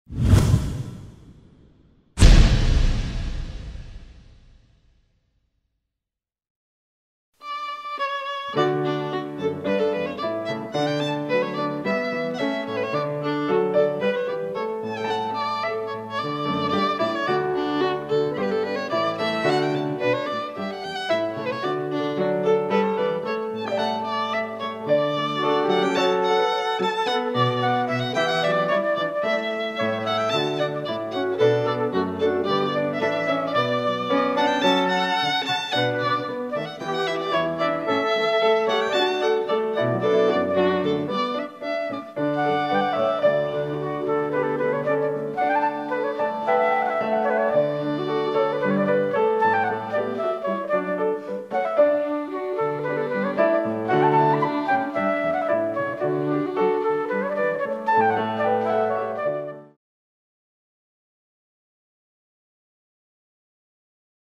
BallTrailer2010a.flv
2010 Jane Austen Ball Trailer #1. The Ball will be held in Rochester, NY on May 2, 2010